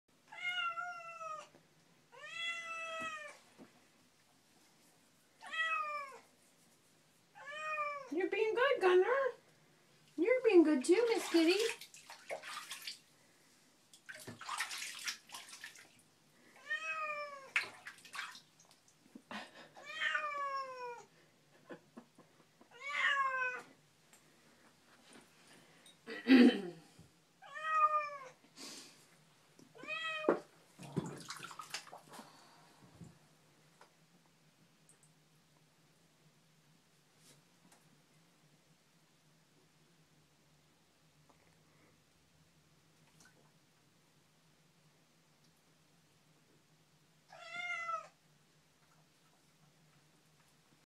Bath time
Kitty and Gunner get a bath